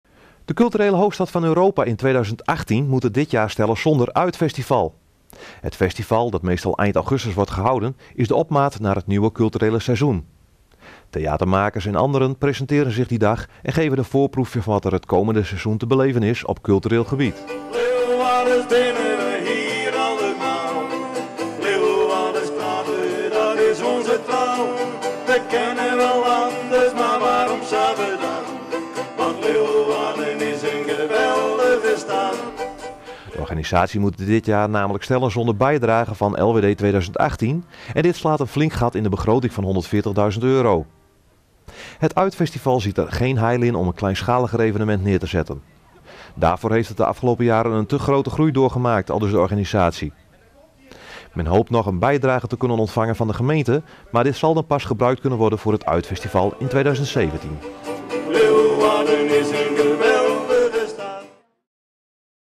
GPTV: Geen Uit!-Festival dit jaar
De Culturele Hoofdstad van Europa in 2018 moet het dit jaar stellen zonder UIT!Festival. Het festival dat meestal eind augustus wordt gehouden, is de opmaat naar het nieuwe culturele seizoen. Theatermakers en anderen presenteren zich die dag en geven een voorproefje van wat er het komende seizoen te beleven is op cultureel gebied. De organisatie moet het dit jaar namelijk stellen zonder bijdrage van LWD 2018 en dit slaat een flink gat in de begroting van 140.000 euro. Het UIT!Festival ziet er geen heil in om een kleinschaliger evenement neer te zetten. Daarvoor heeft het de afgelopen jaren een te grote groei doorgemaakt. De organisatie hoopt nog een bijdrage te kunnen ontvangen van de gemeente, maar dit zal dan pas gebruikt kunnen worden voor het UIT!Festival in 2017.